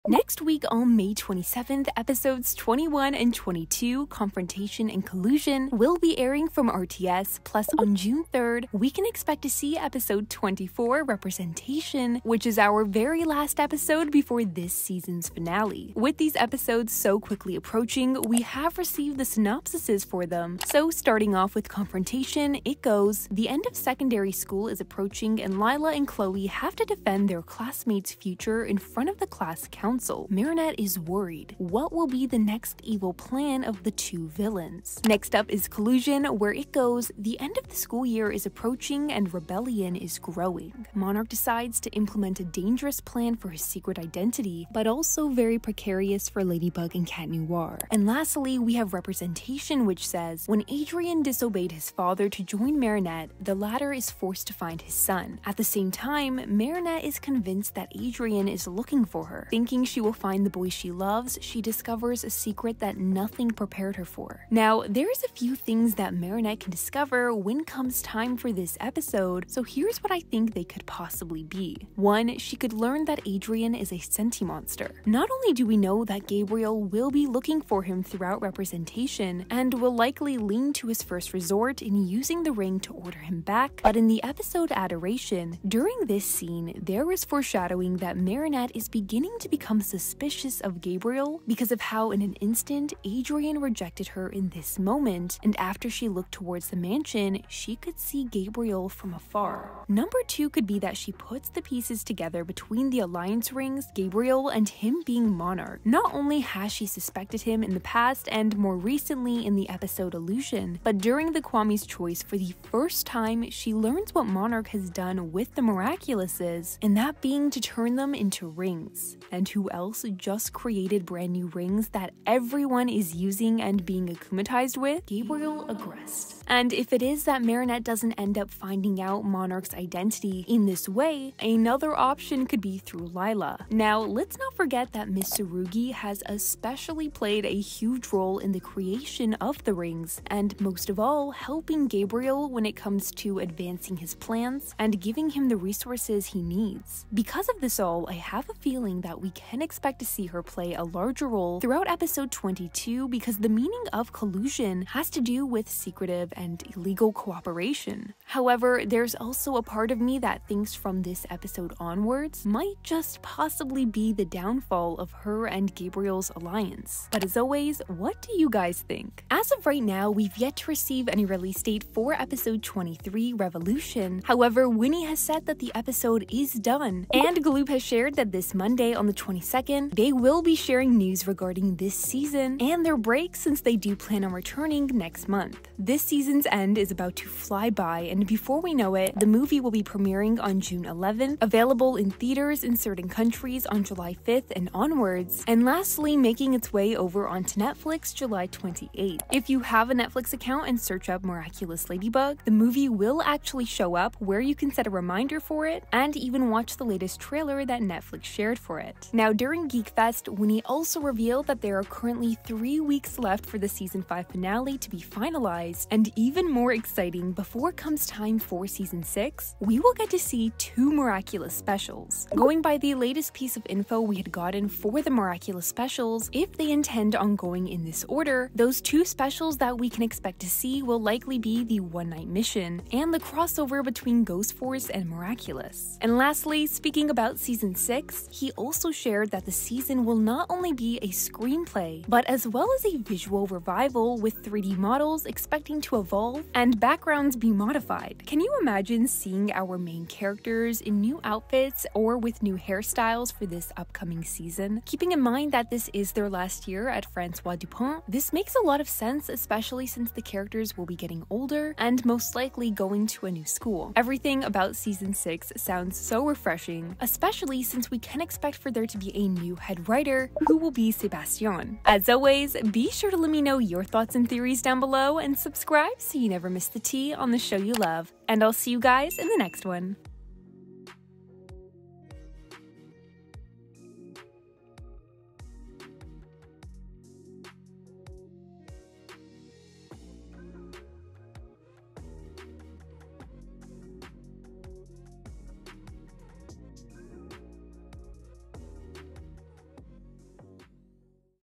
NEW MIRACULOUS LADYBUG SEASON 5 EPISODES 21, 22, AND 24 SYNOPSIS'S +THEORIES! 🐞✨
Check out the brand new Miraculous Ladybug Season 5 synopsis's we've received for episodes 21 Confrontation, 22 Collusion, and 24, our last episode before this season's finale, Representation! 💗

Please do not copy or re-upload my videos or thumbnails on any platform.

THANK YOU  X  167K! 🥰 ✨